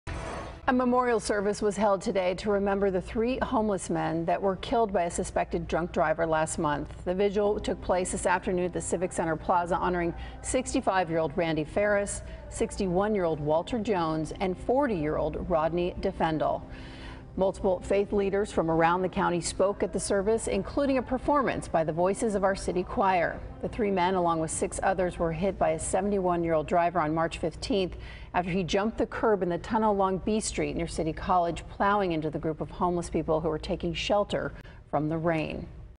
Vigil Held For Homeless Men Killed By Driver
A memorial service was held Sunday to remember the three people killed by a suspected drunk driver last month.